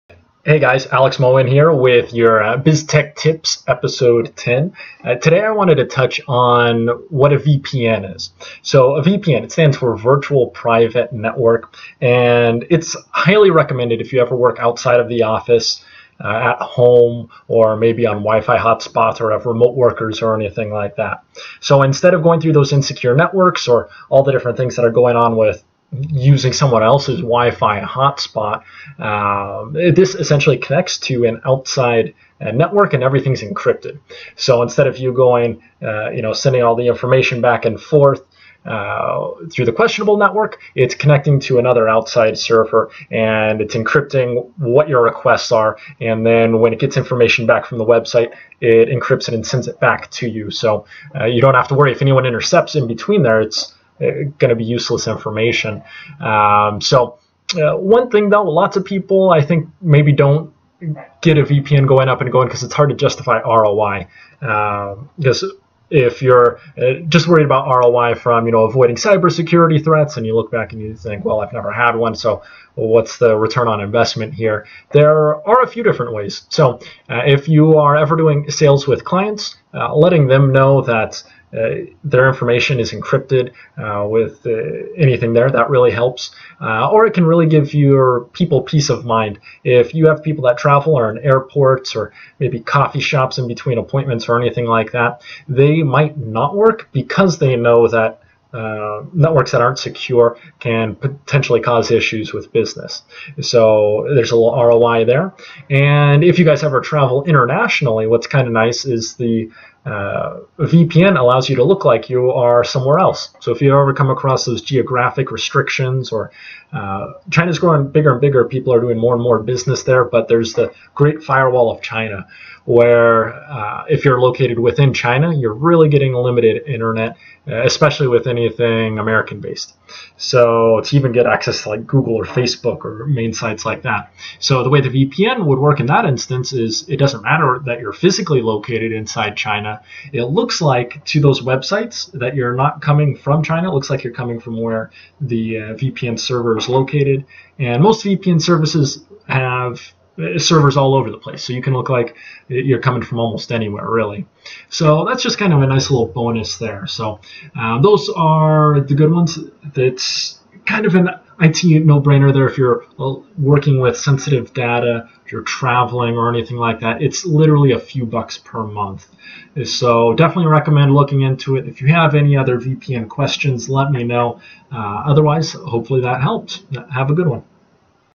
What is a VPN and what is its ROI? :: Biz Tech Tips, episode 10
VPN stands for virtual private network, and it can keep you safe, and is very inexpensive.

If you ever work remotely, or have remote workers, a VPN is highly HIGHLY recommended.

Instead of your Internet connection running through an insecure home network, or a questionable WiFi hot spot, it connects to a private, encrypted network. That network relays your Internet requests to the websites and sends it back to you, all nicely encrypted and with no need to worry, even if people are trying to snoop because it can't be decoded.

The ROI is easy to justify, even if you don't consider potential threats reason enough:

1) A VPN can also be a good sales tool for your clients, as you let them know that their sensitive information is protected with heavy encryption.

2) A VPN can provide peace of mind to you and your employees while you travel, allowing them to feel comfortable to do work when they might have not otherwise.

3) And, on a side note, if you've ever been abroad and found yourself having access restricted due to your geography, this is a handy way to get around that. Business in China (or access to Google, or Facebook, or to many other American sites the Chinese government blocks), for instance, is not a problem at all with a VPN because it looks like you are located wherever the VPN is setup and not where you're physically located, and many VPN companies have locations all over the world. So, in the China example, you can be physically located in China but look like you're actually connecting to websites from the US due to your VPN.

There are tons of other benefits, whether you use a VPN for yourself personally, or whether you get one for your whole company. At a cost of just a few bucks per month, it can be an IT no-brainer for many people

#1 Life Changing Book

If you are on the west coast, book a free call with me to see if we can help